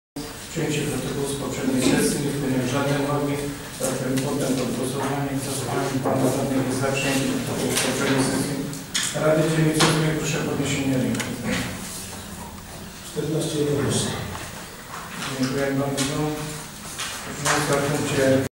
RD_VIII_18-12-2013_2 Przyjęcie protokołu XLVI Sesji
Obrady XLII sesji Rady Dzielnicy VIII Dębniki z dn. 19 grudnia 2013.